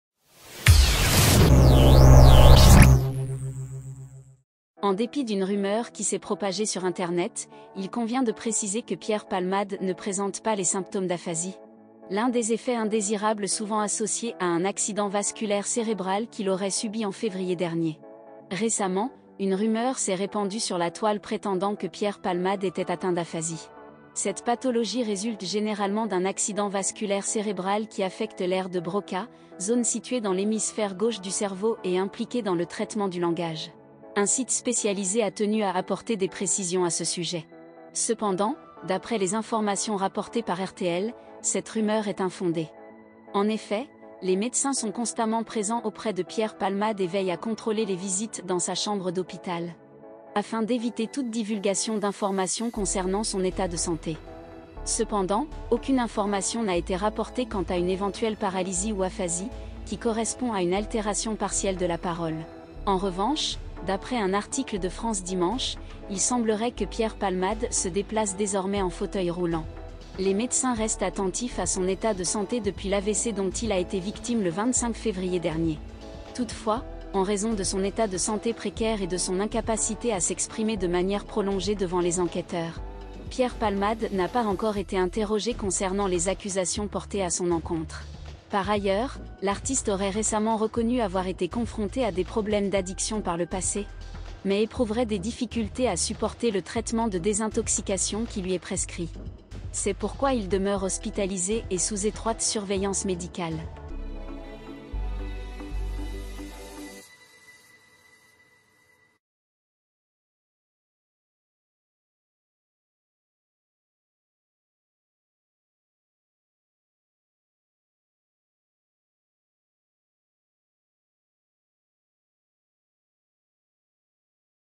Pierre Palmade aphasique, les mystères sur son état de santé révélés
En dépit d'une rumeur qui s'est propagée sur internet, il convient de préciser que Pierre Palmade ne présente pas les symptômes d'aphasie, l'un des effets indésirables souvent associés à un accident vasculaire cérébral qu'il aurait subi en février dernier.

Récemment, une rumeur s'est répandue sur la toile prétendant que Pierre Palmade était atteint d'aphasie. Cette pathologie résulte généralement d'un accident vasculaire cérébral qui affecte l'aire de Broca, zone située dans l'hémisphère gauche du cerveau et impliquée dans le traitement du langage. Un site spécialisé a tenu à apporter des précisions à ce sujet.

Cependant, d'après les informations rapportées par RTL, cette rumeur est infondée. En effet, les médecins sont constamment présents auprès de Pierre Palmade et veillent à contrôler les visites dans sa chambre d'hôpital, afin d'éviter toute divulgation d'informations concernant son état de santé.

Cependant, aucune information n'a été rapportée quant à une éventuelle paralysie ou aphasie, qui correspond à une altération partielle de la parole. En revanche, d'après un article de France Dimanche, il semblerait que Pierre Palmade se déplace désormais en fauteuil roulant. Les médecins restent attentifs à son état de santé depuis l'AVC dont il a été victime le 25 février dernier.

Toutefois, en raison de son état de santé précaire et de son incapacité à s'exprimer de manière prolongée devant les enquêteurs, Pierre Palmade n'a pas encore été interrogé concernant les accusations portées à son encontre.

Par ailleurs, l'artiste aurait récemment reconnu avoir été confronté à des problèmes d'addiction par le passé, mais éprouverait des difficultés à supporter le traitement de désintoxication qui lui est prescrit. C'est pourquoi il demeure hospitalisé et sous étroite surveillance médicale.